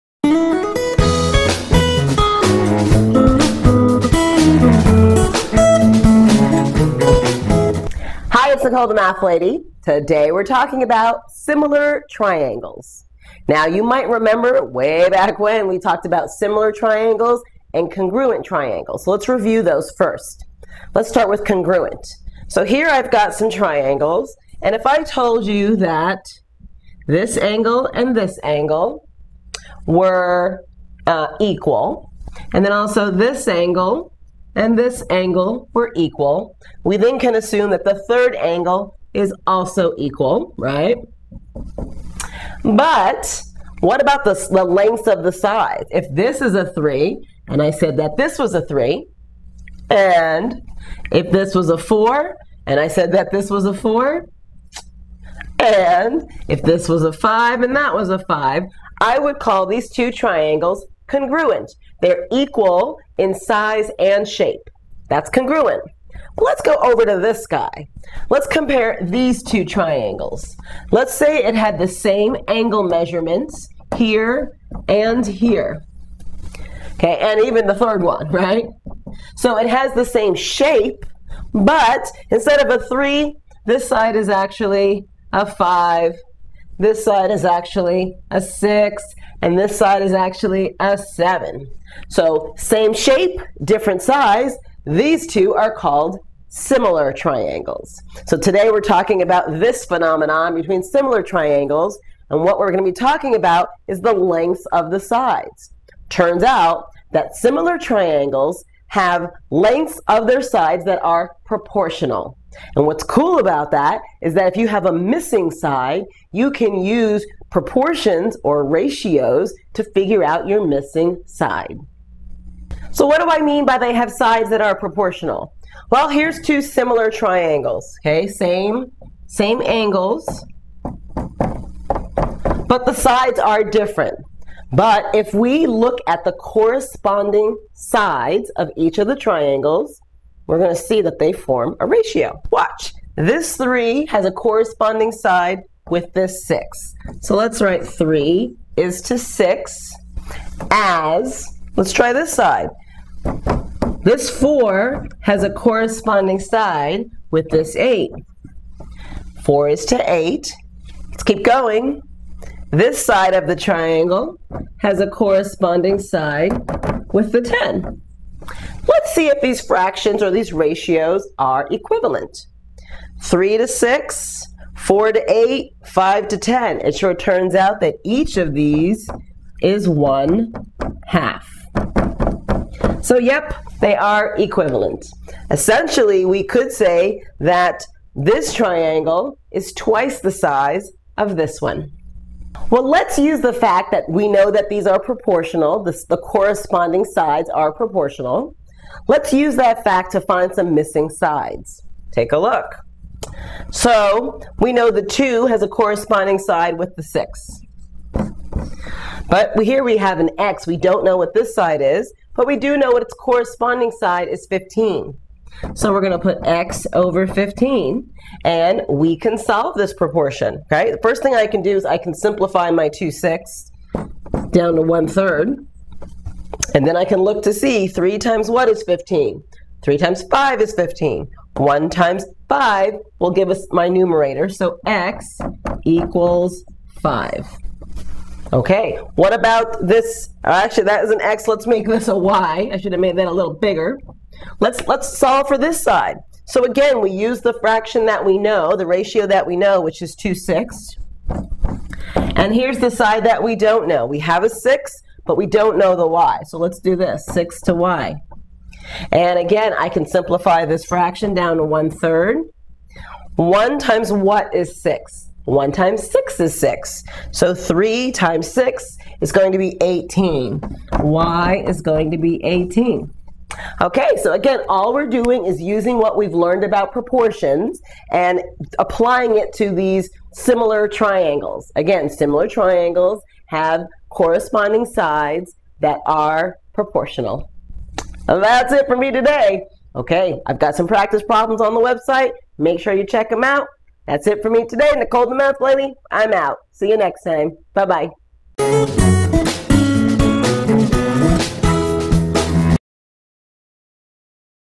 97a. Similar Triangles (for Saxon Math 8/7)
Watch Nicole teach her version of Lesson 97 of the Saxon Math 8/7 Curriculum. For the full set of lessons (odd and even) and practice sets for every lesson,